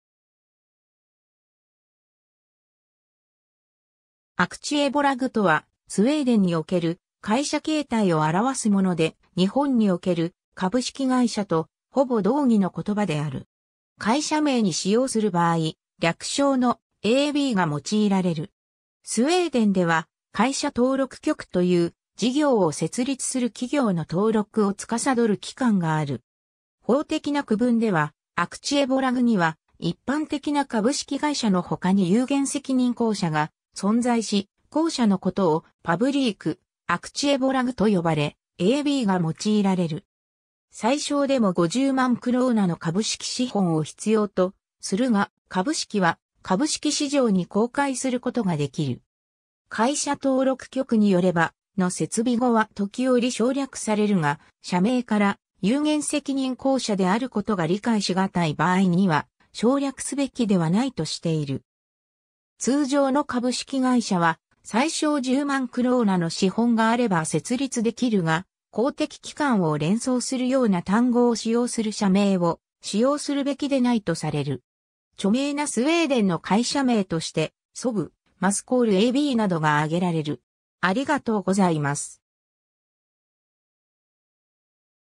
アクチエボラグ
アクチエボラグ(瑞:aktiebolag)とは、スウェーデンにおける会社形態を表すもので、日本における株式会社とほぼ同義の言葉である。
会社名に使用する場合、略称の「AB」が用いられる。
スウェーデンでは会社登録局(Bolagsverket)という、事業を設立する企業の登録を司る機関がある。
 法的な区分では、アクチエボラグには一般的な株式会社のほかに有限責任公社が存在し、後者のことをパブリーク・アクチエボラグ(瑞:publikt aktiebolag)と呼ばれ「AB(publ)」が用いられる。
最小でも50万クローナ(約630万円)の株式資本を必要とするが、株式は株式市場に公開することができる。
会社登録局によれば、「(publ)」の接尾語は時おり省略されるが、社名から有限責任公社であることが理解しがたい場合には省略すべきではないとしている。
 通常の株式会社は最小10万クローナ(約130万円)の資本があれば設立できるが、公的機関を連想するような単語を使用する社名を使用するべきでないとされる。
 著名なスウェーデンの会社名としてSAAB(Svenska Aeroplan AB)、MySQL ABなどが挙げられる。